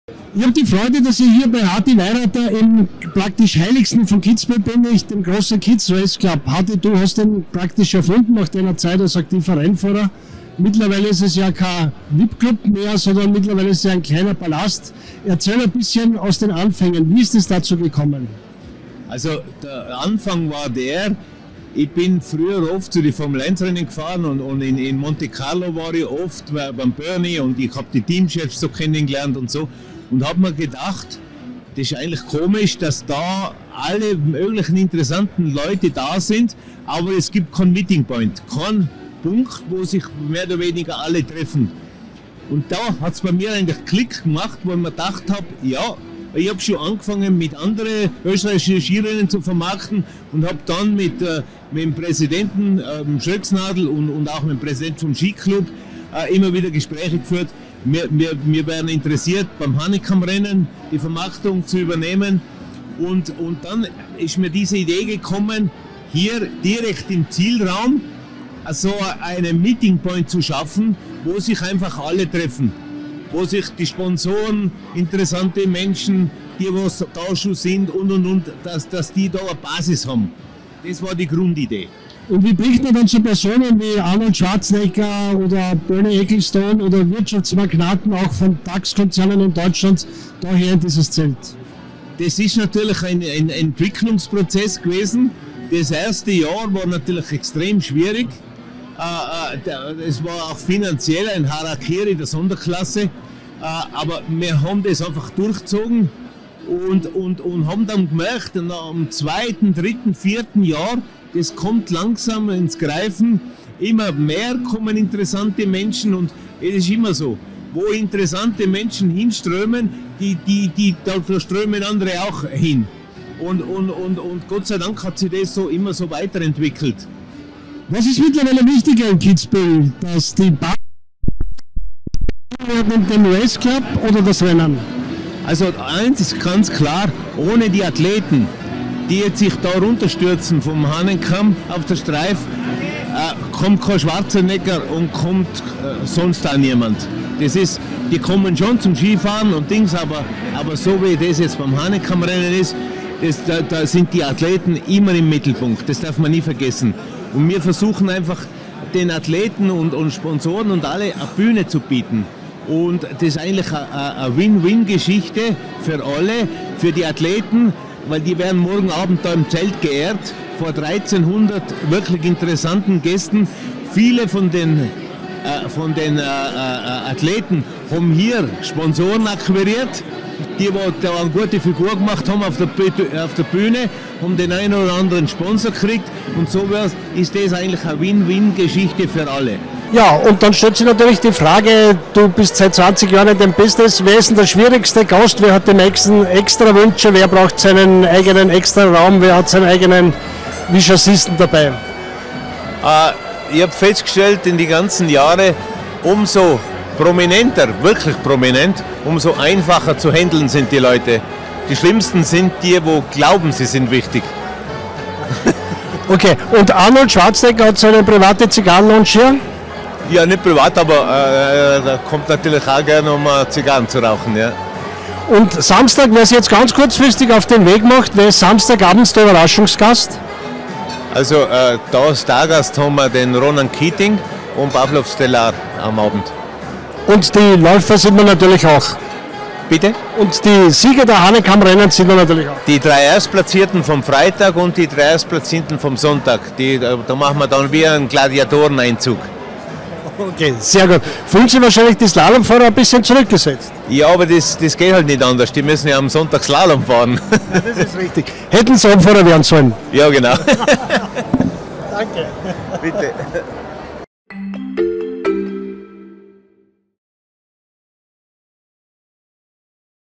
Harti Weirather im Interview zur Entstehung des Kitz Race Clubs
Der ehemalige österreichische Skirennläufer Harti Weirather erzählt im SN-Interview aus Kitzbühel von der Entstehung des Kitz Race Club, an der er maßgeblich beteiligt war.